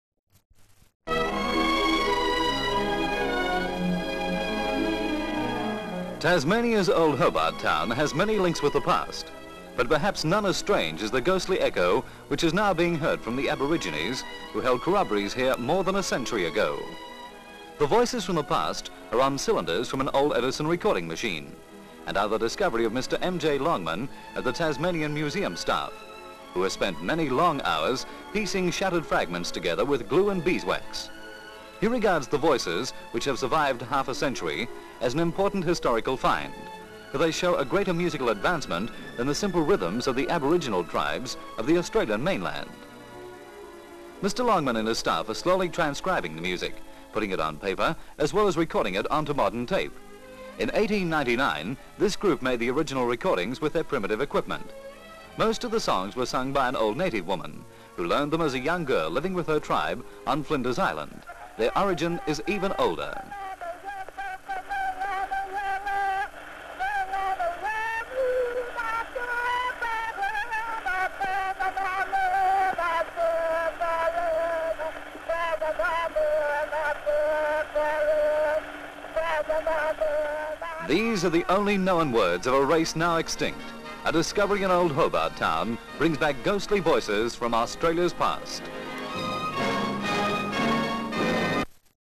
Rare Recordings Of Extinct Tribe (1964)
Title reads: "Tasmania. Rare Recording of Extinct Tribe". Australian voiceovered newsreel material.

Hobart,Tasmania, Australia.

VS of Hobart. VS In the Tasmanian Museum the staff at work fixing together ancient cylinder records of old aboriginal tribal songs. They play them back on old gramophone and record them on tape recorder.

(Comb.F.G.) Old record suggests material dates from around 22/04/1964.
 FILM ID:3105.36